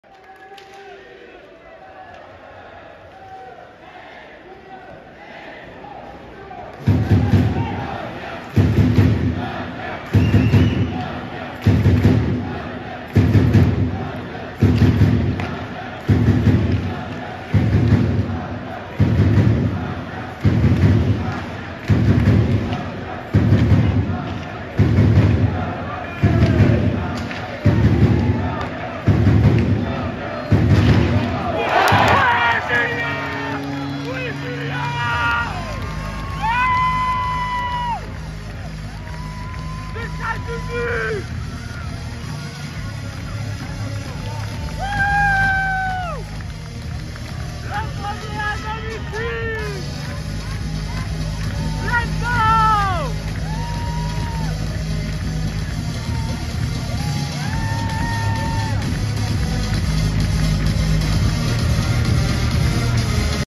Julien Tessier But (Amiens vs Anglet) - Ligue Magnus - 08/10/2023 (8-5)
BUUUUUT POUR AMIENS !!! Un but inscrit par le numéro 19 Julien Tessier !!! Son premier but au Coliséum !!!